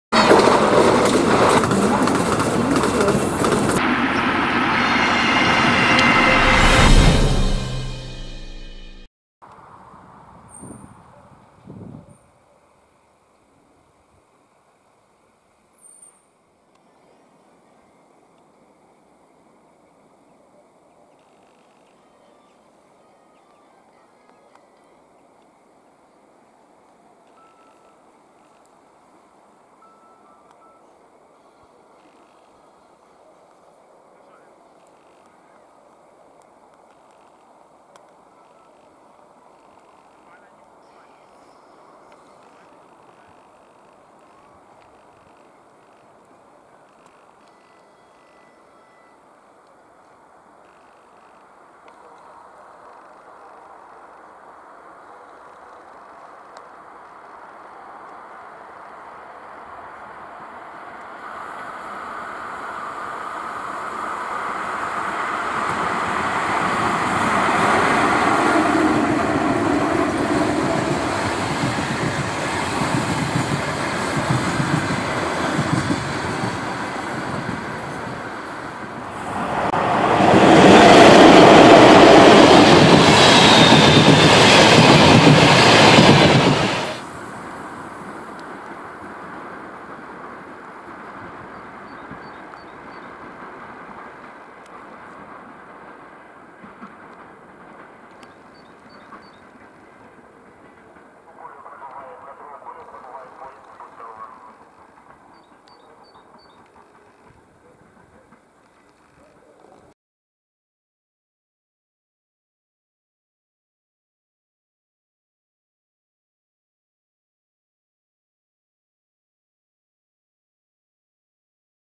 Карантин! ЧС8-010 со служебным поездом
Киев. Поезд сообщением Тетерев - Дарница. Снято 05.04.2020 г. Служебный поезд прибывает на станцию Беличи.